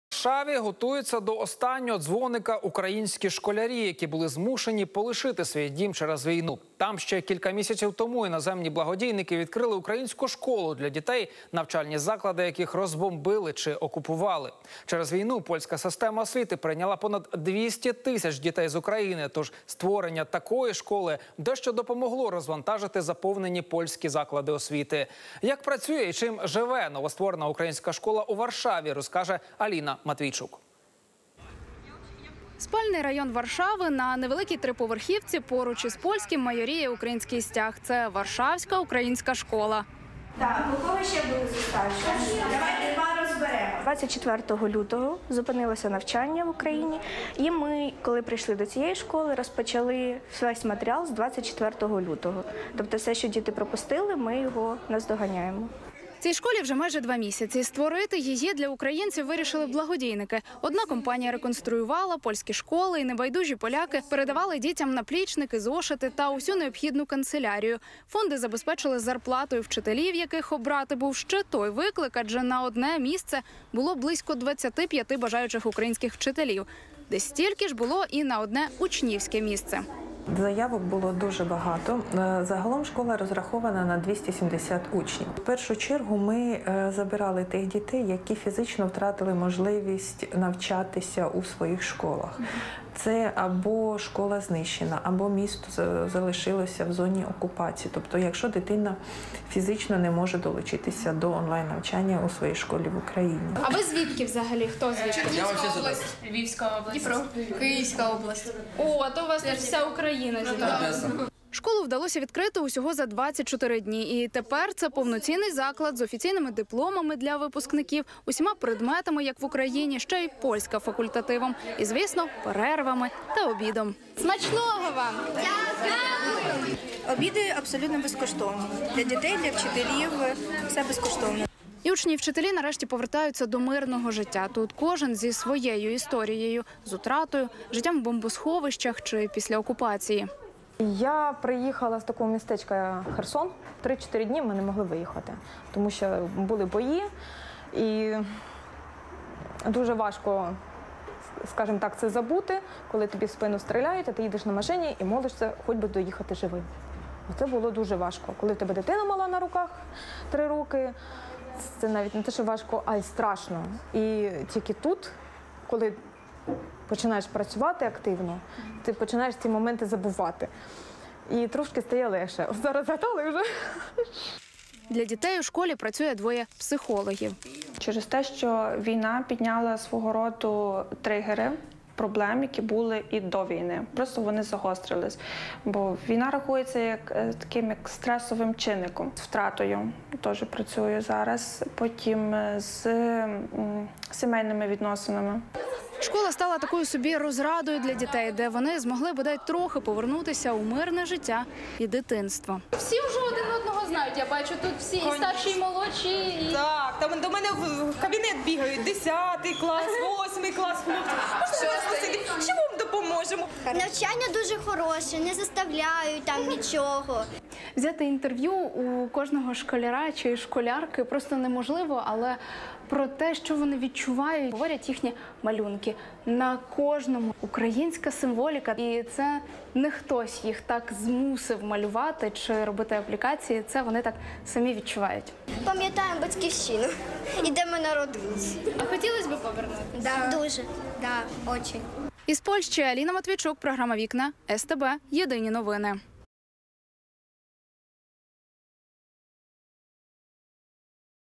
🔔 Українські діти готуються до випускного у Польщі - як працює і чим живе новостворена школа
У Варшаві готуються до останнього дзвоника українські школярі, які були змушені полишити свій дім через війну.

Там ще кілька місяців тому іноземні благодійники відкрили українську школу для дітей, навчальні заклади яких розбомбили чи окупували.

Через війну Польська система освіти прийняла понад 200 тисяч дітей з України, тож створення такої школи дещо допомогло розвантажити заповнені польські заклади освіти.

Як працює і чим живе новостворена українська школа у Варшаві, розкаже Аліна Матвійчук.

Побач більше!